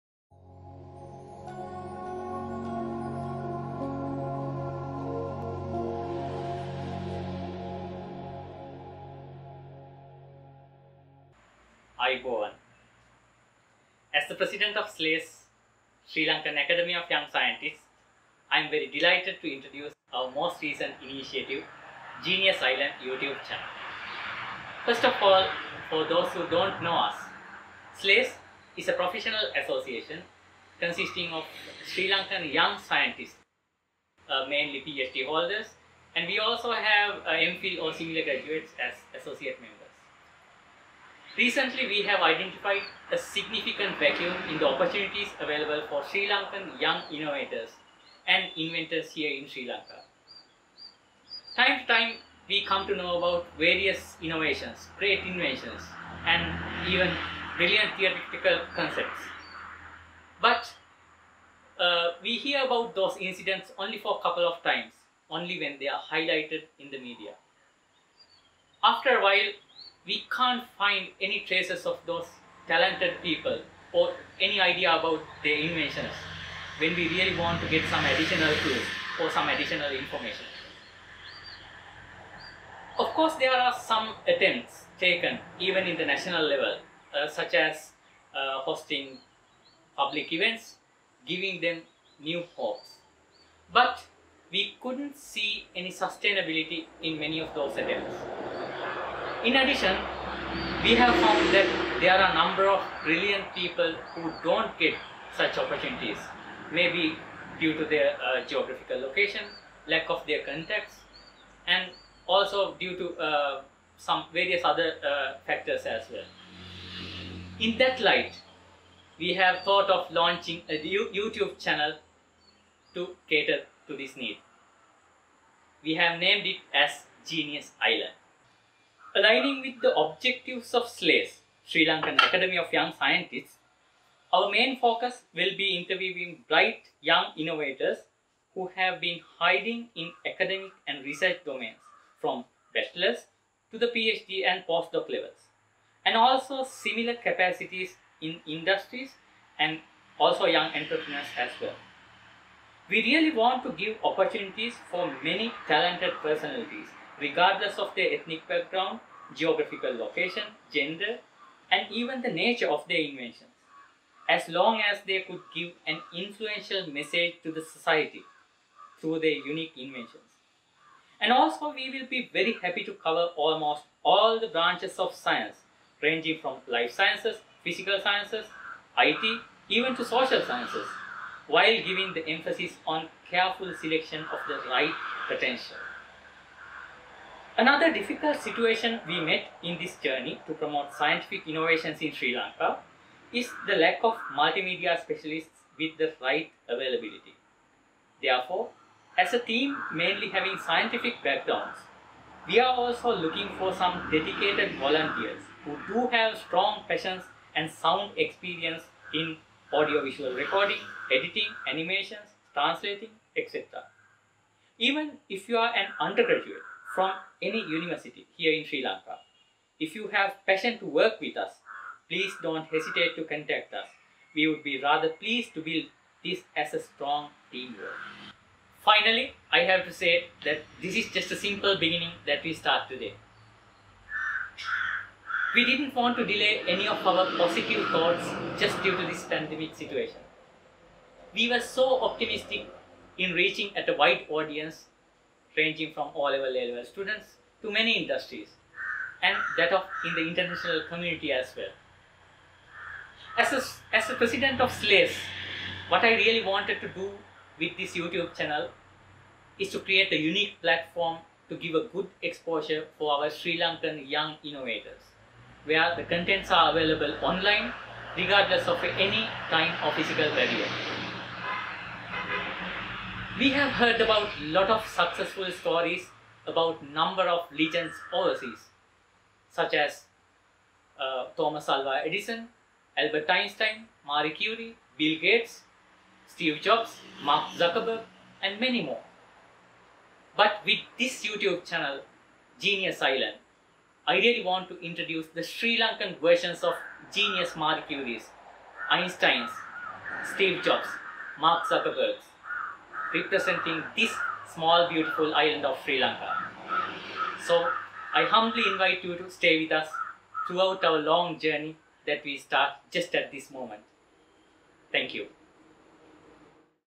Introduction to ❝Genius Island by SLAYS❞ – Dr. Asanka Pallewatta
❝Genius Island❞ YouTube Channel is a unique platform initiated by SLAYS to give good exposure for our Sri Lankan innovators. Aligning with the objectives of Sri Lankan Academy of Young Scientists (SLAYS), our main focus will be interviewing bright young innovators who have been hiding in academic and research domains from bachelors to the PhD and postdoc levels. Also to showcase the potentials in industries, and young entrepreneurs as well. We really want to give opportunities for many talented personalities, regardless of their ethnic background, geographical location, gender, and even the nature of their invention, as long as they could give an influential message to society through their unique inventions.

Presenter / Concept – Dr. Asanka Pallewatta
Video post-production – Anupama Rathnayaka and Isuru Samarasinghe / RedDot Productions.